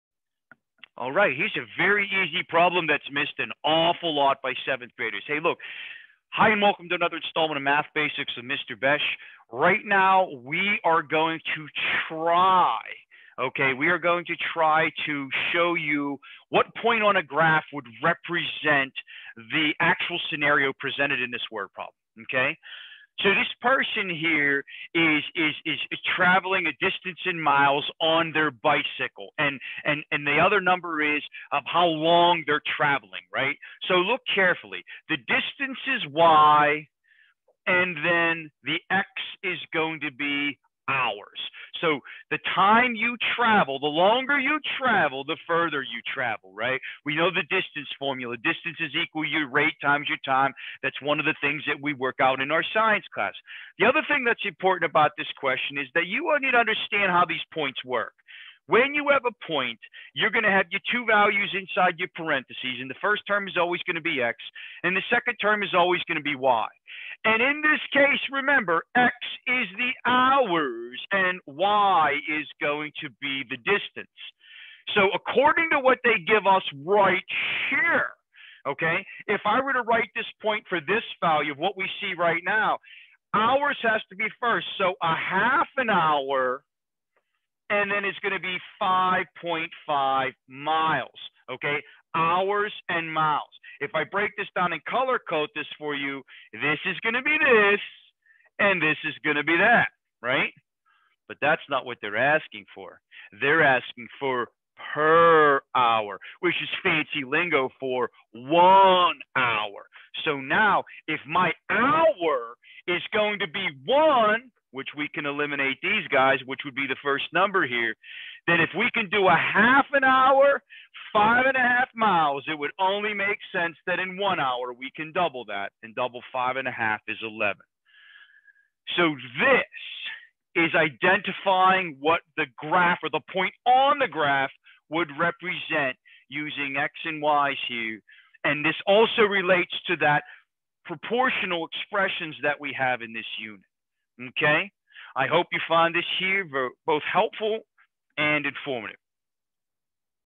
Word Problem to Ordered Pair
This video describes how one would set up an ordered pair being described from a word problem... Rate/Unit Rate.